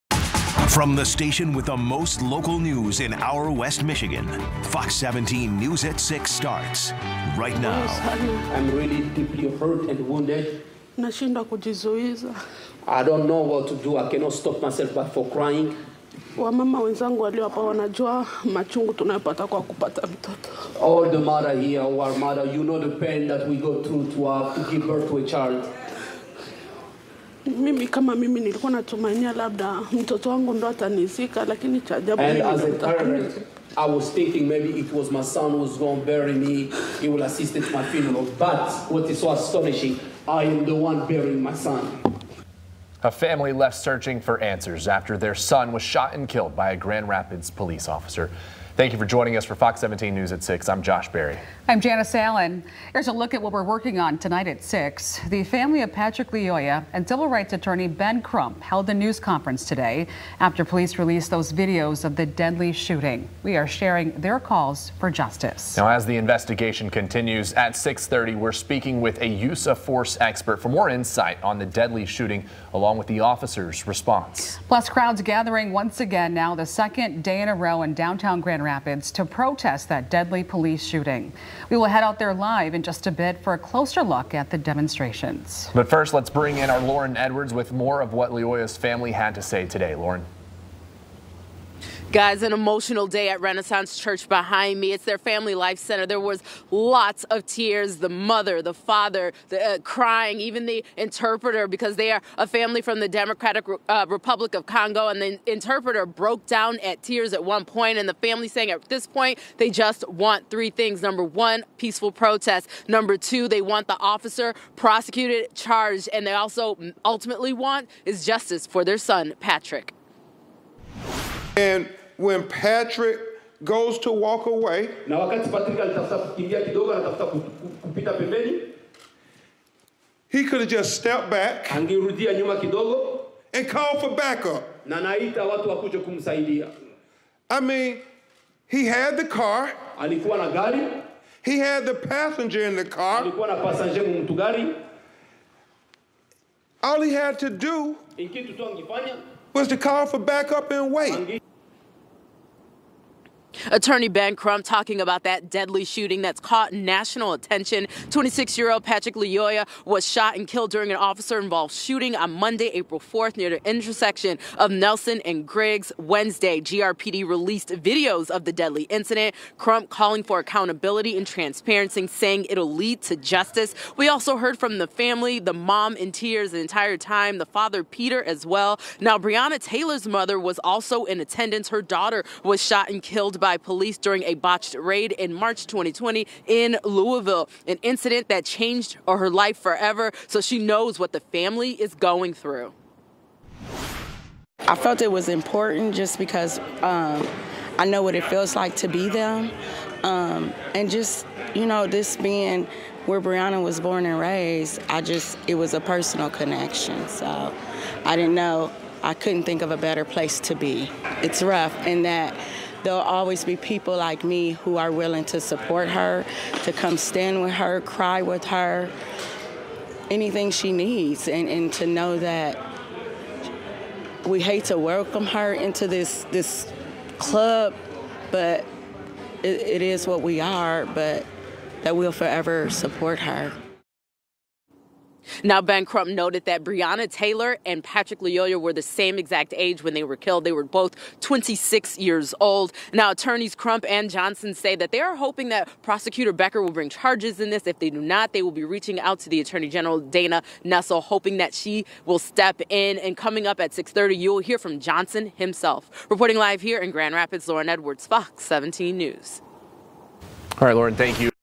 ‘My heart is really deeply broken:’ Lyoya family, Crump continue calls for justice
‘My heart is really deeply broken:’ Lyoya family, Crump continue calls for justice at press event on Thursday at Renaissance Church.